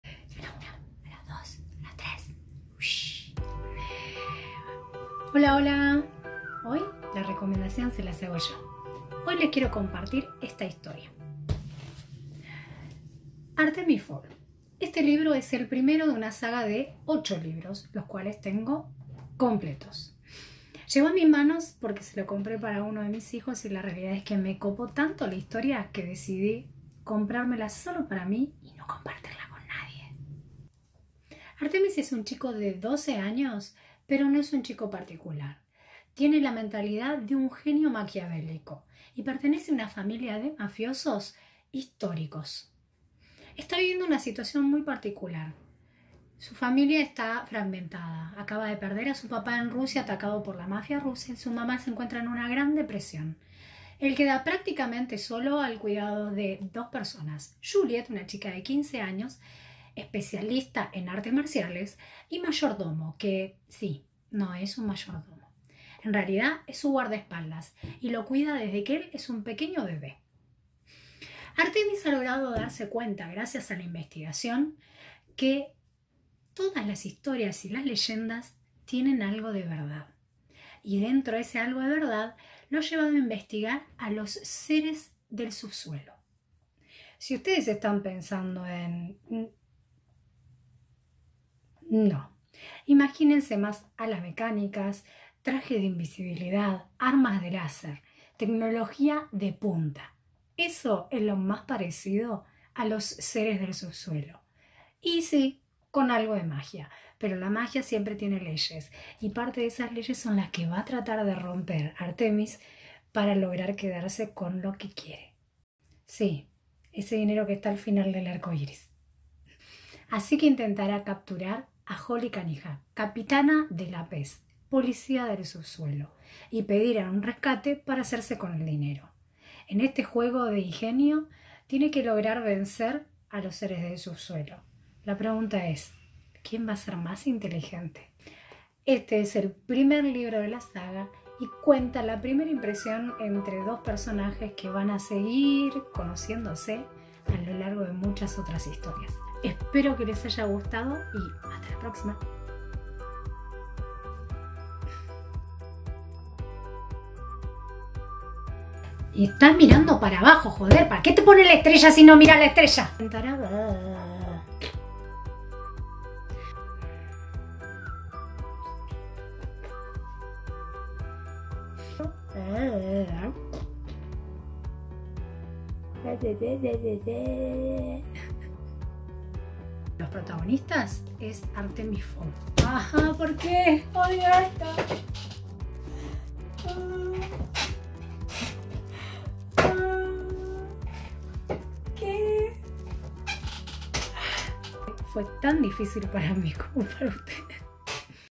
Videoreseña literaria: "Artemis Fowl"
Reseña literaria sobre el libro "Artemis Fowl" de Eoin Colfer
Desde mi perspectiva lectora cuento sin spoiler uno de los libros que más me gusta...

La idea... volverme booktuber por un ratito :)